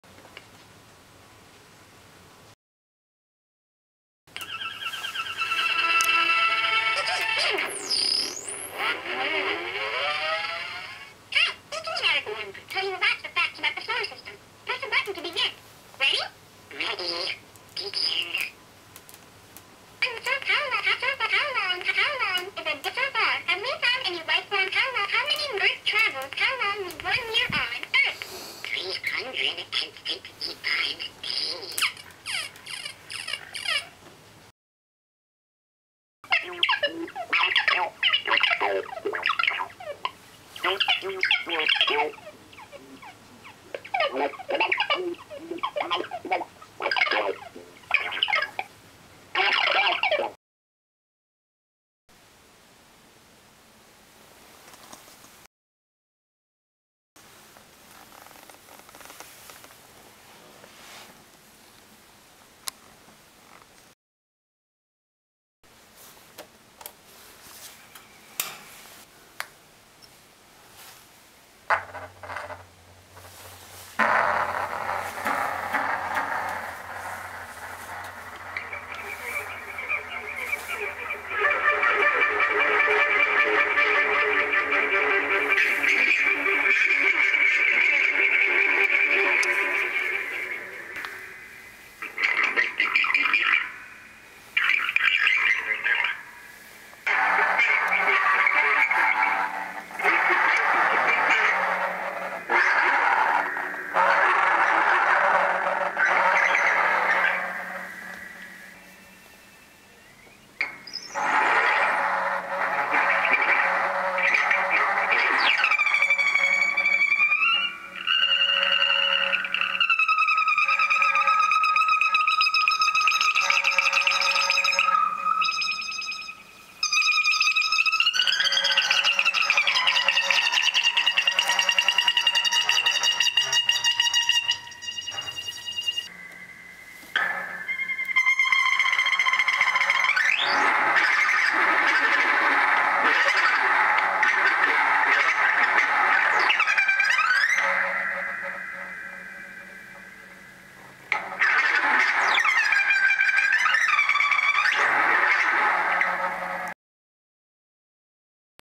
ET extra terrestrial communication device for 1st 2nd and 3rd contact 2012 circuit bent Atlantis
I, for one, will welcome the imminent arrival of our grey and somewhat moist overlords with this translation device of their own provenance. Modified to wring the secret language of the universe from mundane 'facts' about the 'solar system'. Clock and bandpass add-ons, with 'theremin' photoresistors, and dual LED oscillators that drove the goosenecks, just like they found at Roswell.